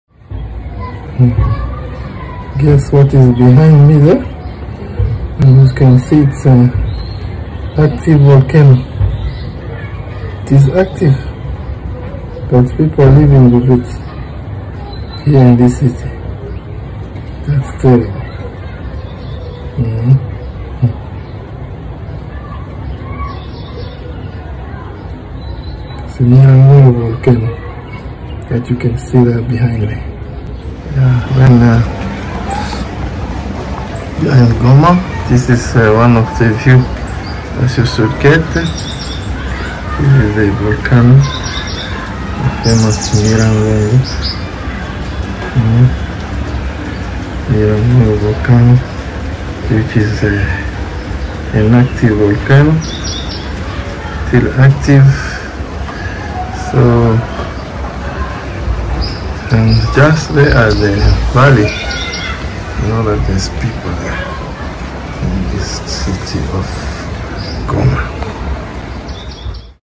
A Travers Goma (Across Goma) Part 4: Nyiragongo Volcano in the Day
Nyiragongo Volcano 🌋 in Goma, eastern DRC. An active volcano with smoke escaping from the crater. But people still living near it. Last eruption was in 2000 which destroyed almost half of the city. When is the next eruption ?? 🤔 Scientists are monitoring. So, fear not! 😁